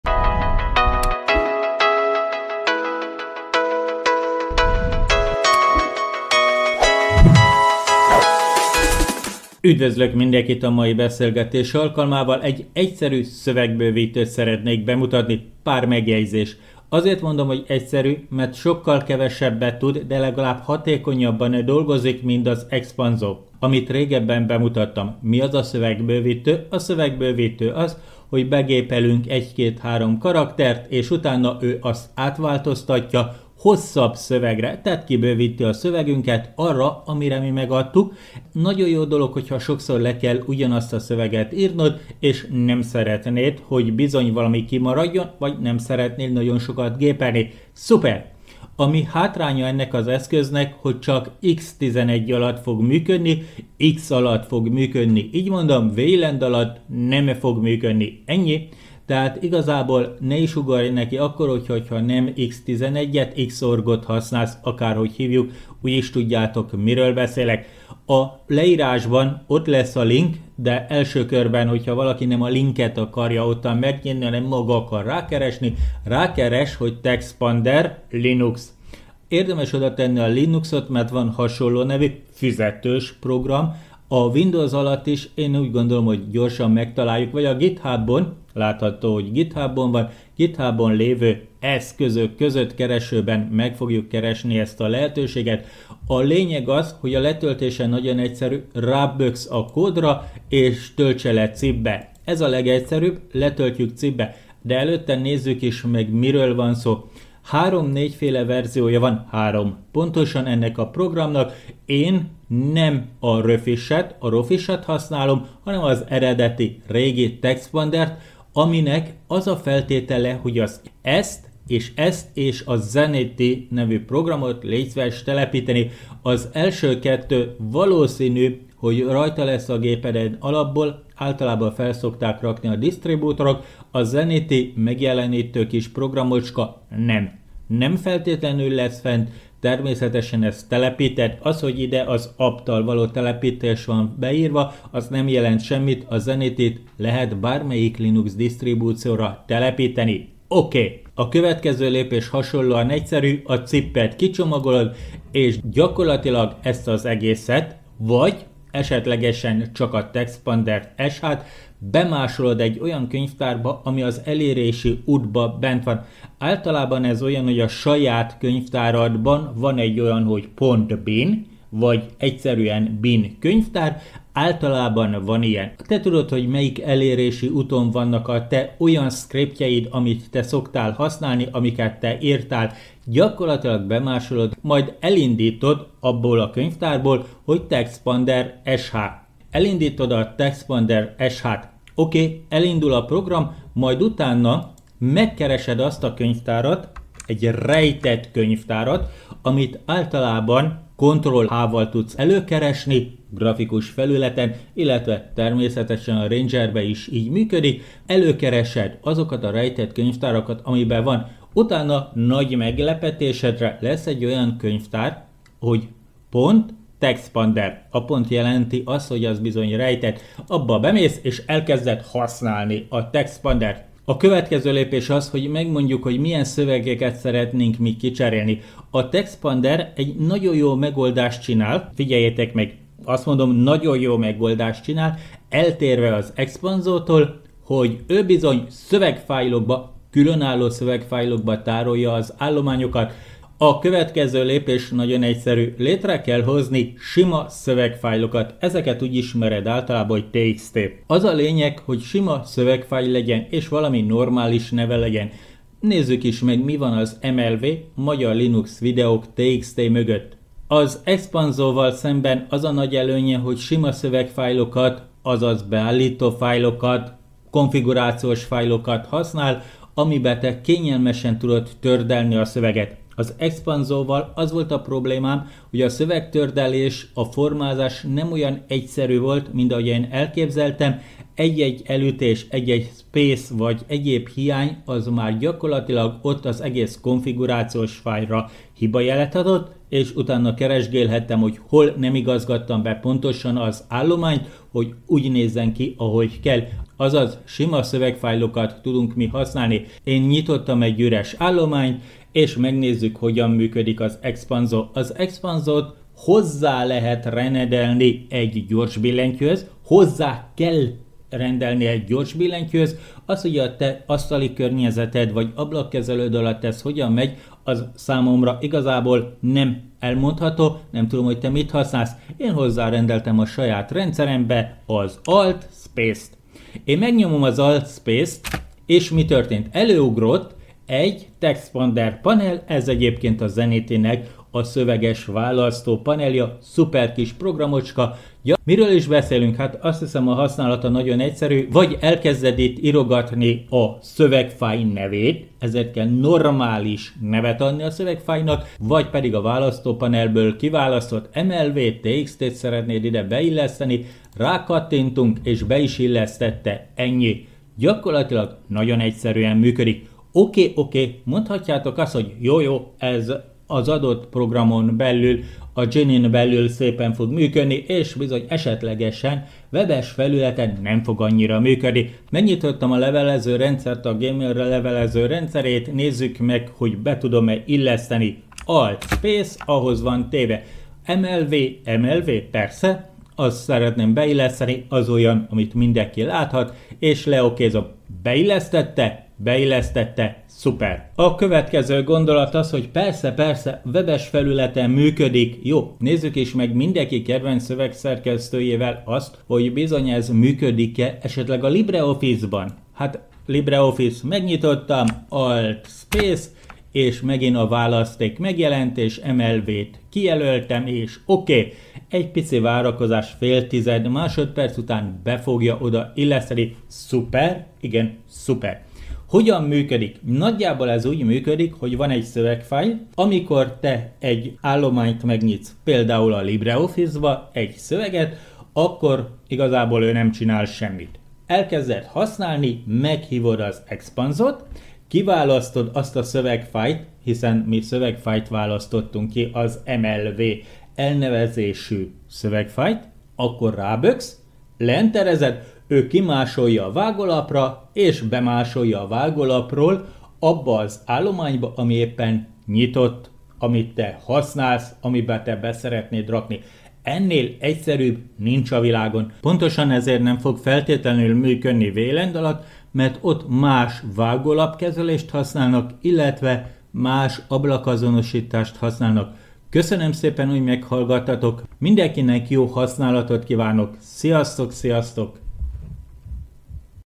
Texpander Linuxos szövegbővítő
A texpander az egyik legegyszerűbben használható szövegbővítő eszköz Linuxra. Gyakorlatilag előre megadott hosszabb szövegeket helyez el - másol be -  a dokumentumokba. Működik webes felületen, és az összes általam kipróbált szövegszerkesztőben:

Jó Linuxozást!

Ha tetszett a téma, akkor lájkold, így tudom, hogy mi az ami érdekes számodra!